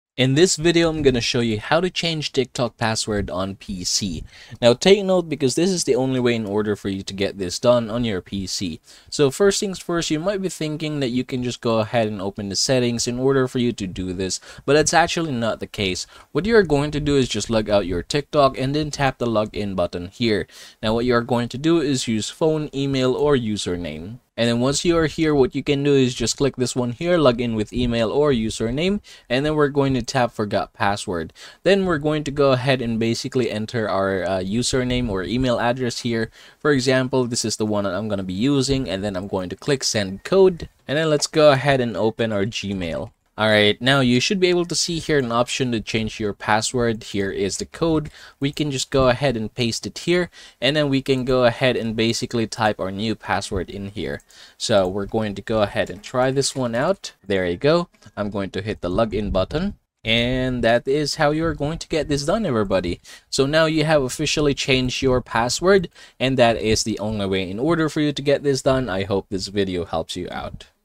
How To Change TikTok Password on PC (Step By Step)
In this video, I show you how to change TikTok Password on PC. Did this video help? :)